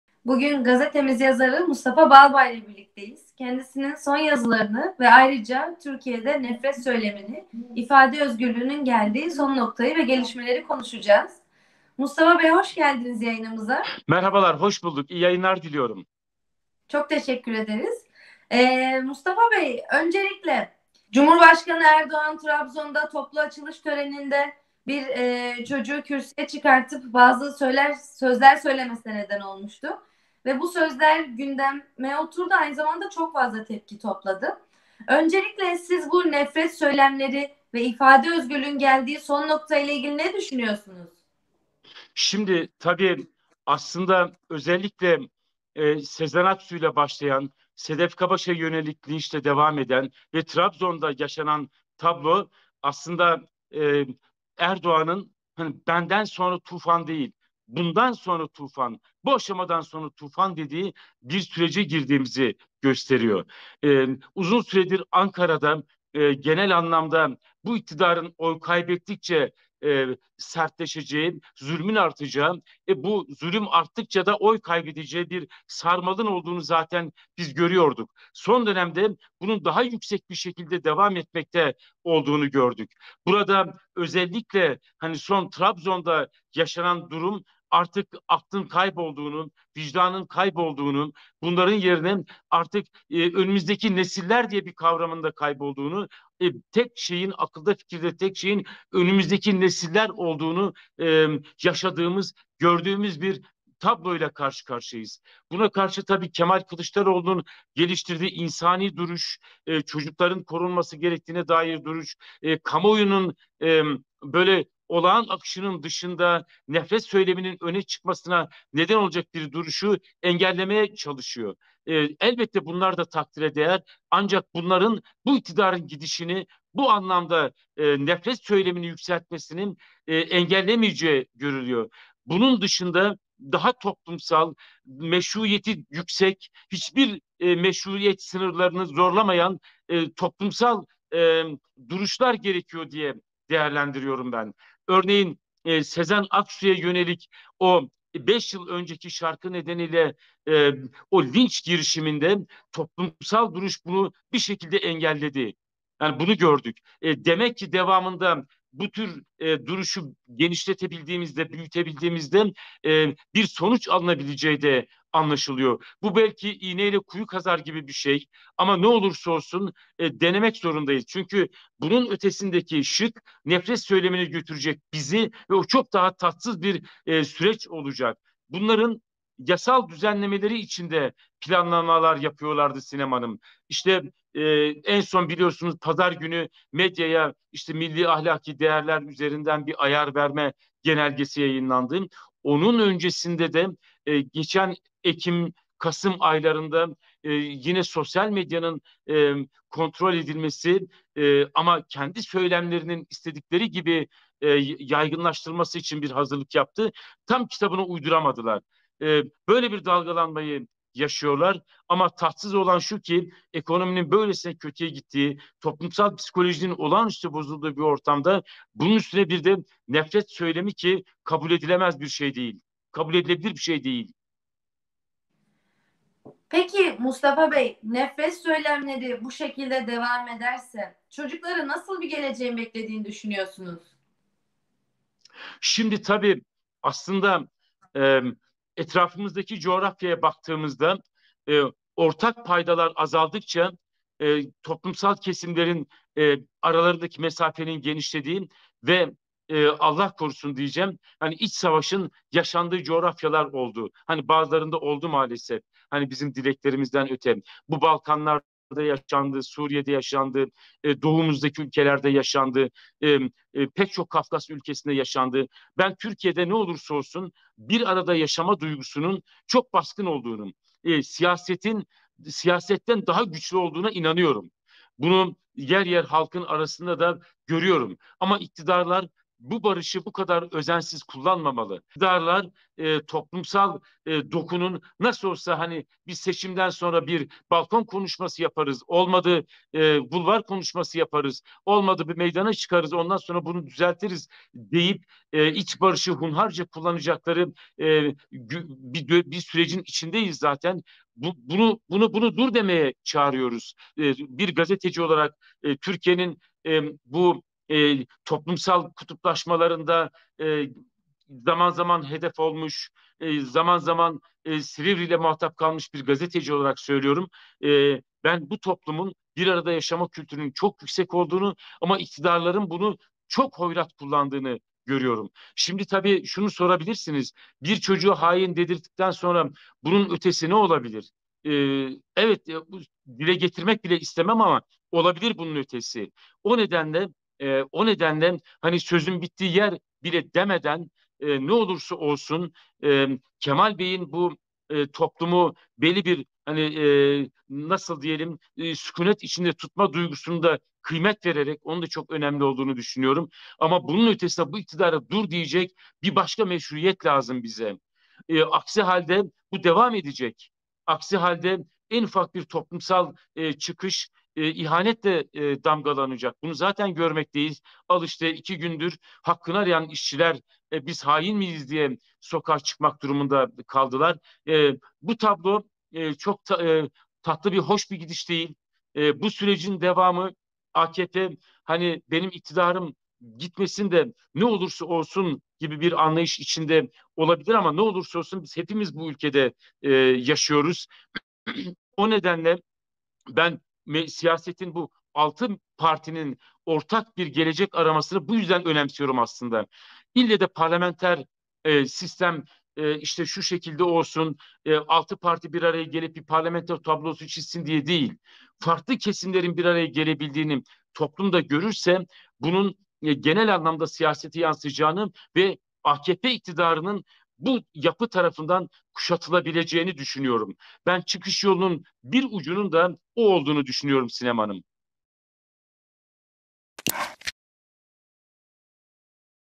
Ankara kulislerinde neler konuşuluyor? AKP'nin hamlelerinin altında yatan gerçek ne?
Gazetemiz yazarı Mustafa Balbay, Cumhurbaşkanı Erdoğan'ın kürsüye küçük bir çocuğu çıkarması ve sonrasında yaşanan gelişmelerle ilgili dikkat çekici açıklamalarda bulundu. Cumhuriyet TV'ye konuşan Balbay, Türkiye'de artan nefret söylemini ve basın özgürlüğüne yöneltilen tehditlerin ayrıntılarını anlattı.